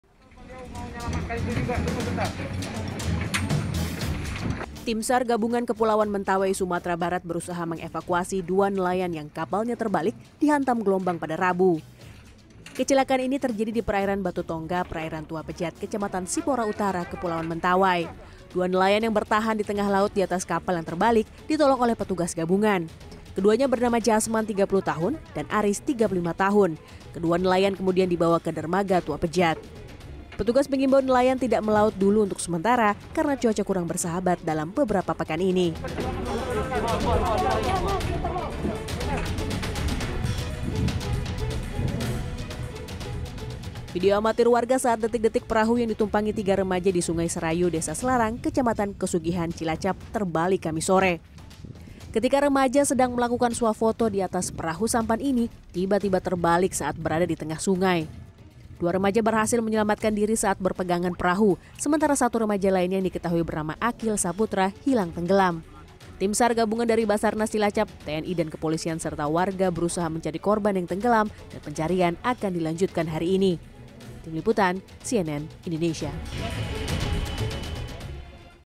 Perahu Terbalik Dihantam Gelombang Tinggi
Tim SAR gabungan kepulauan Mentawai, Sumatera Barat, berusaha mengevakuasi 2 nelayan yang kapalnya terbalik dihantam gelombang pada Rabu. Kecelakaan ini terjadi di perairan Batu Tongga, perairan Tua Pejat, kecamatan Sipora Utara, kepulauan Mentawai.

Dua nelayan yang bertahan di tengah laut di atas kapal yang terbalik, ditolong oleh petugas gabungan. Keduanya bernama jasman, 30 tahun, dan aris, 35 tahun. Kedua nelayan kemudian dibawa ke dermaga tuapejat.

Petugas mengimbau nelayan tidak melaut dulu untuk sementara, karena cuaca kurang bersahabat dalam beberapa pekan ini.

Like & Follow Facebook:
CNN Indonesia

cnnindonesiatv